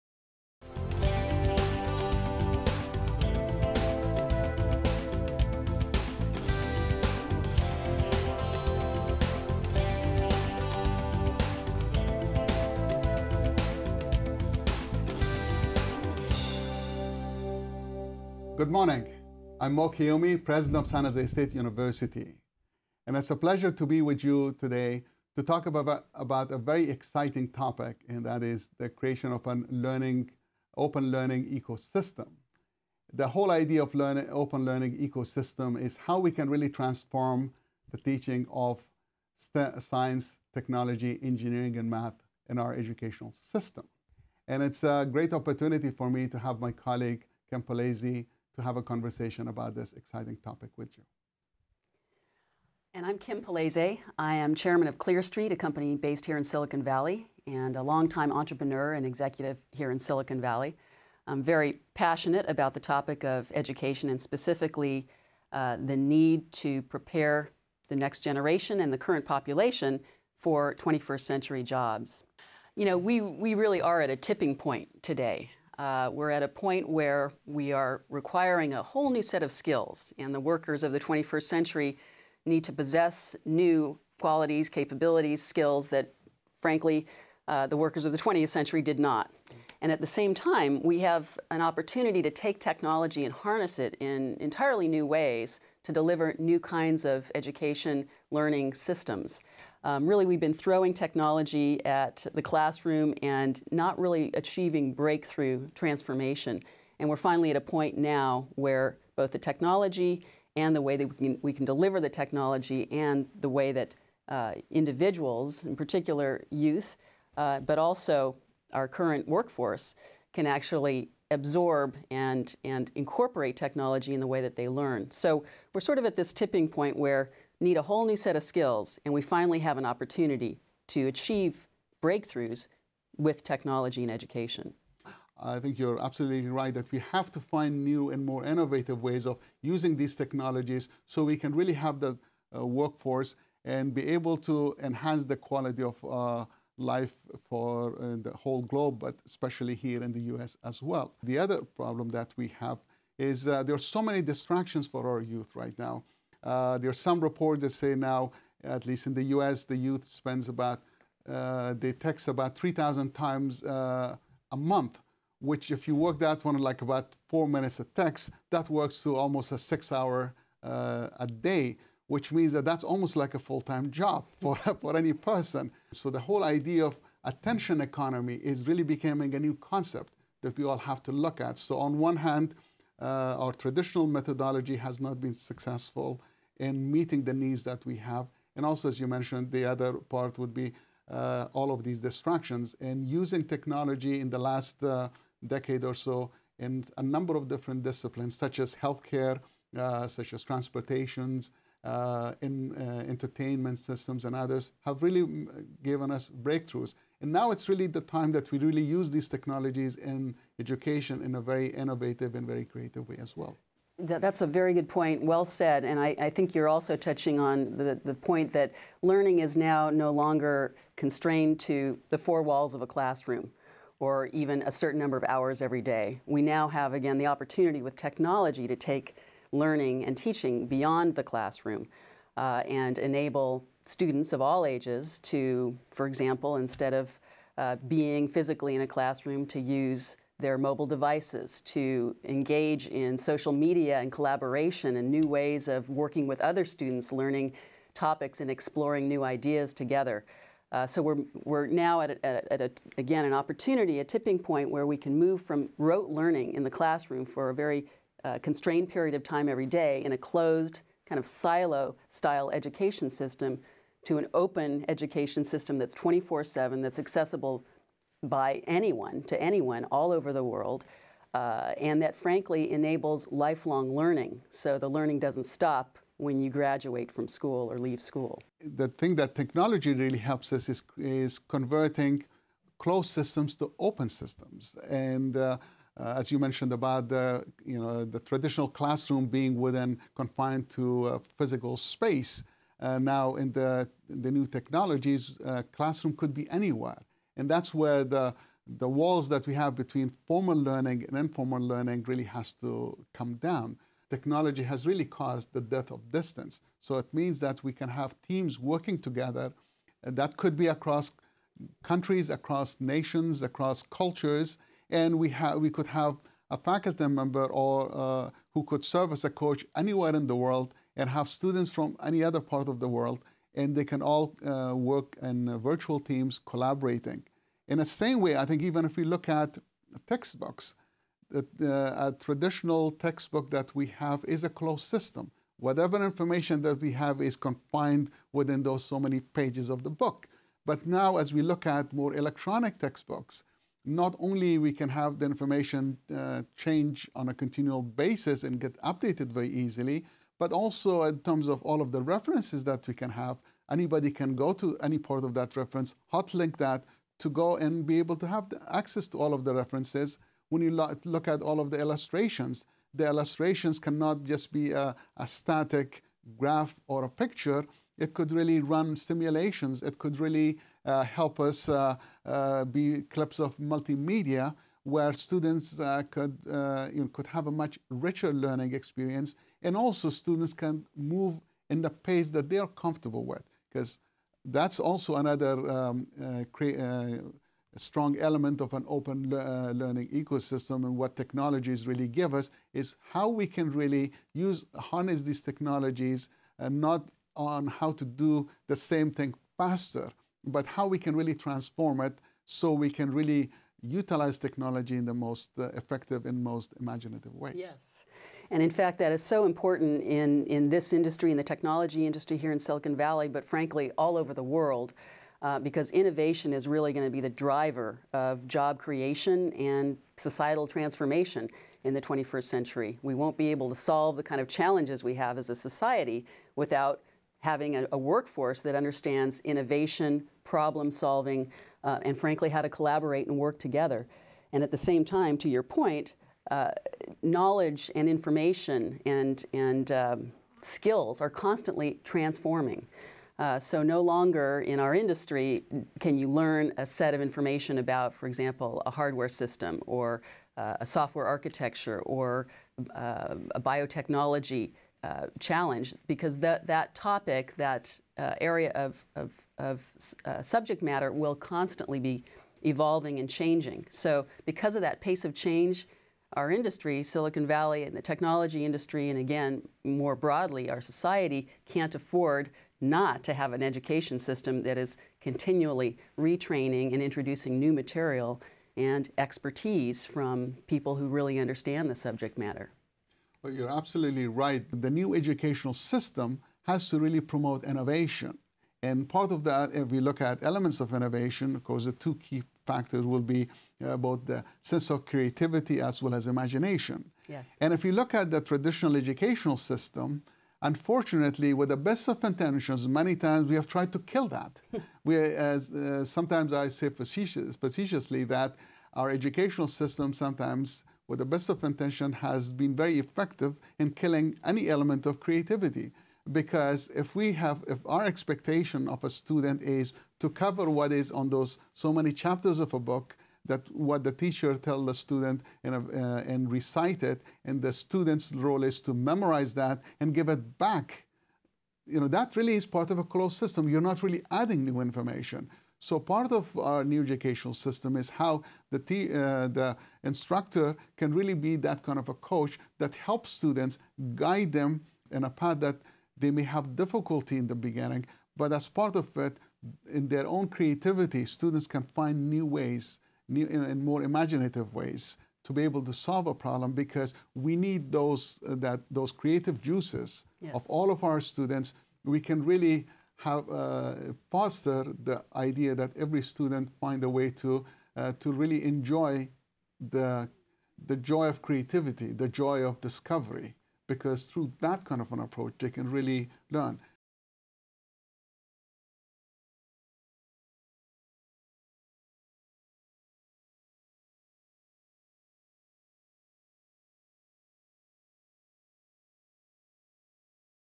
The Open Learning Ecosystem: Transforming Education through the Virtual STEM University- Part 1
A Conversation with Mohammad Qayoumi and Kim Polese